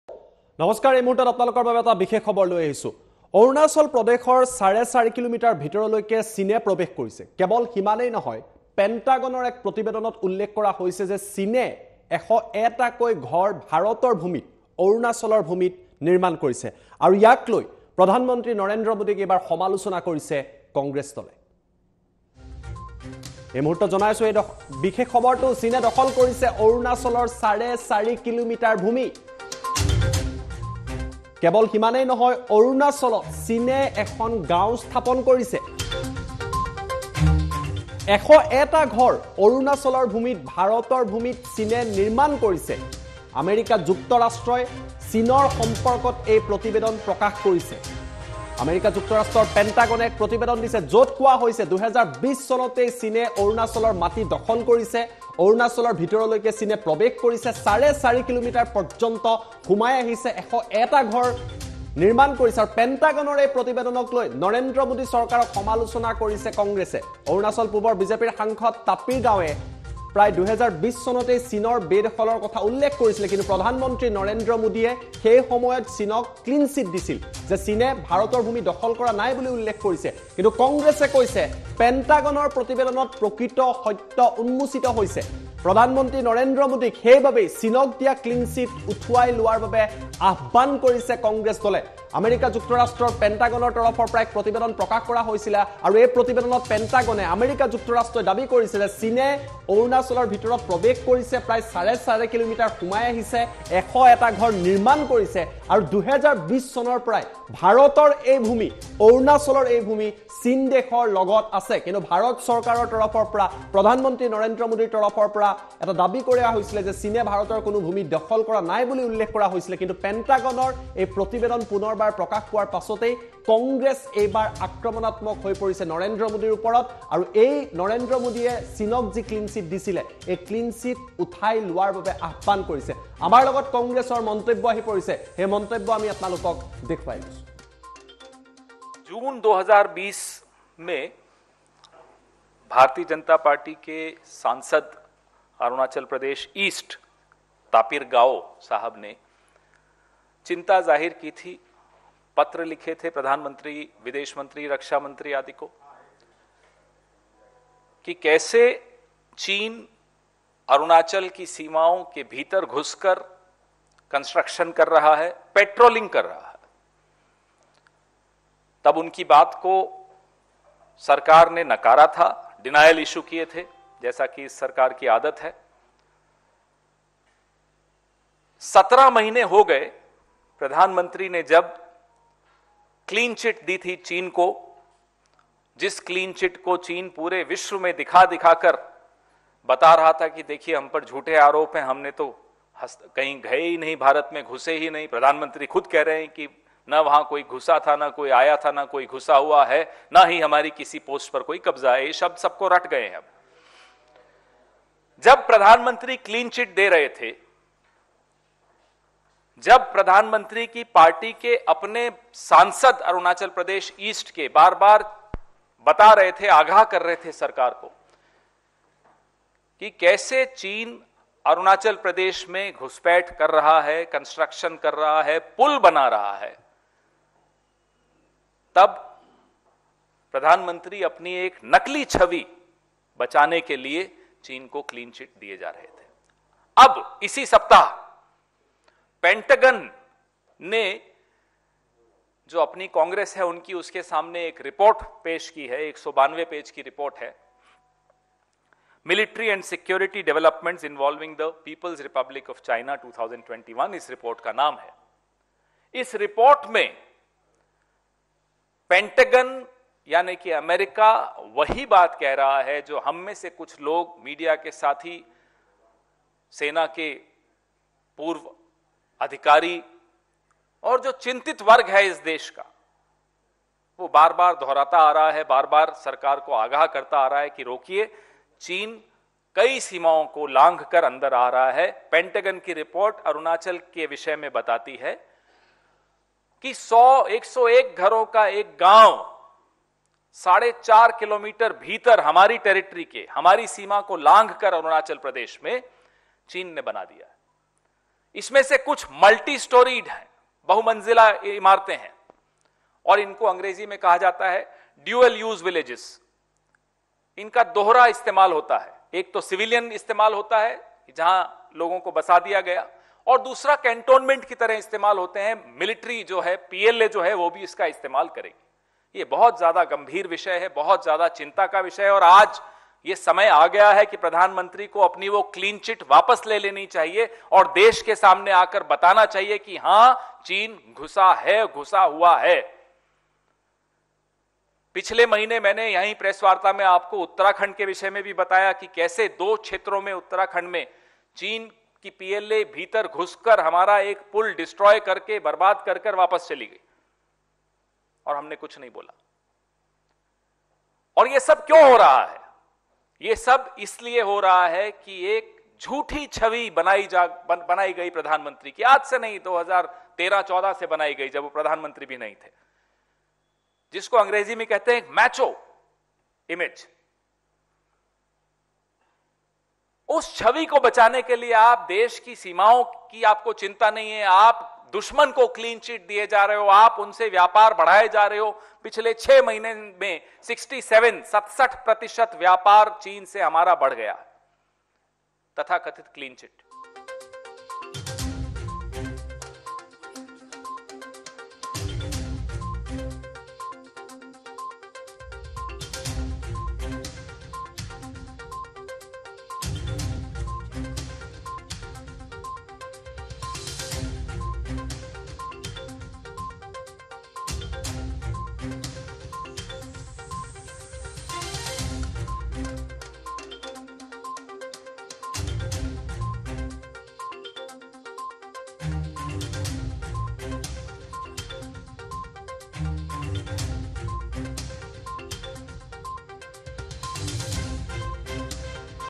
অৰুণাচল প্ৰদেশৰ চাৰে চাৰিকিলোমিটাৰ ভিতৰলৈ সোমাই আহিল চীন
অৰুণাচল প্ৰদেশৰ চাৰে চাৰিকিলোমিটাৰ ভিতৰলৈ সোমাই আহিল চীন। স্থাপন কৰিলে চীনে গাঁও। নিৰ্মাণ কৰিলে ১০১টা বাসগৃহ। আমেৰিকাৰ পেণ্টাগনৰ এক প্ৰতিবেদন প্ৰকাশ পোৱাৰ পিছতেই প্ৰধান মন্ত্ৰী নৰেন্দ্ৰ মোদীক ক্ষুৰধাৰ সমালোচনা কংগ্ৰেছৰ। চীনক মোদীয়ে দিয়া ক্লীনশ্বিট উঠাই লোৱাৰ দাবী কংগ্ৰেছ দলৰ।